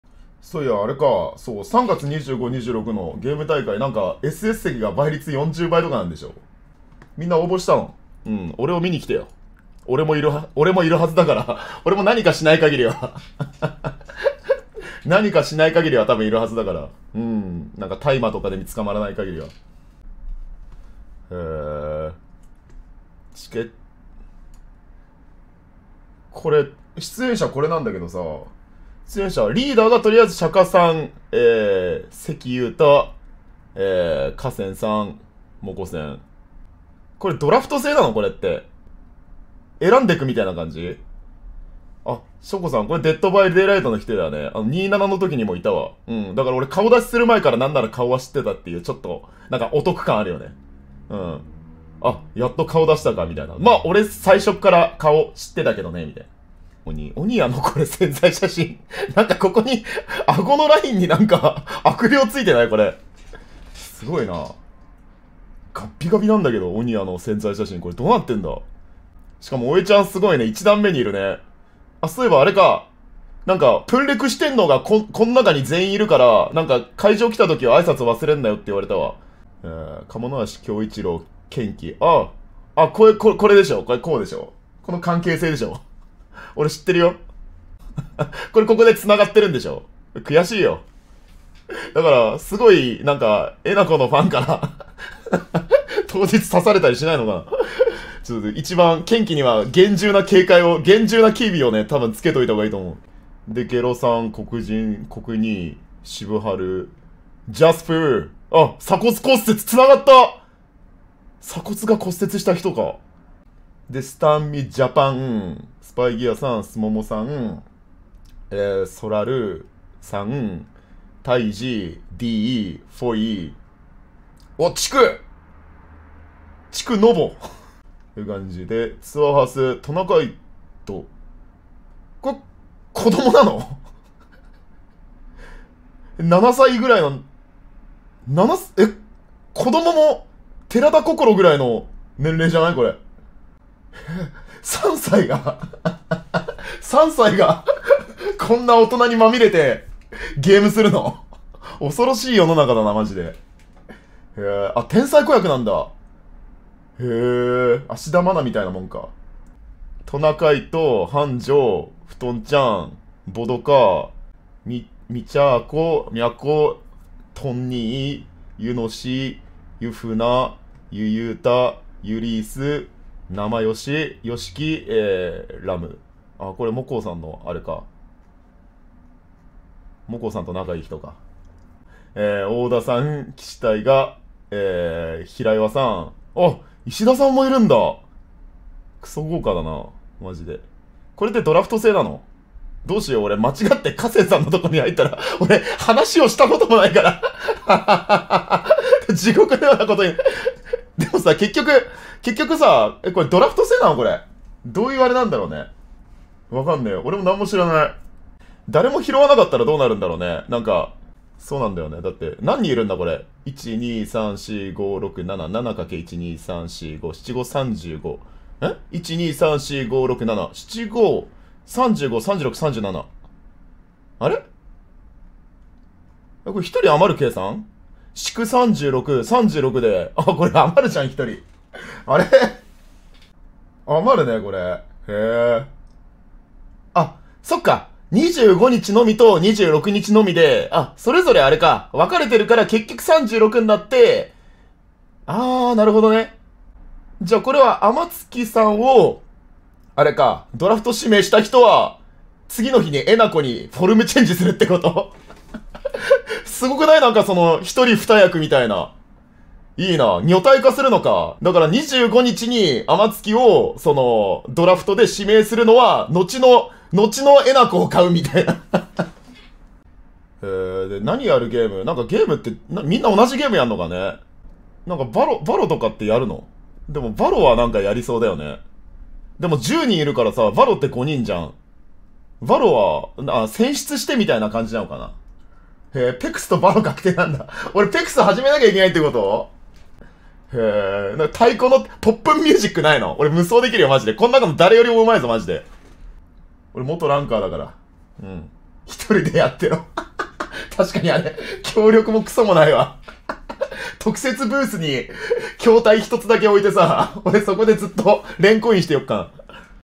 「配信者ハイパーゲーム大会」の出場者を見るゆゆうた【2023/02/05】
元配信
ゆゆうた押忍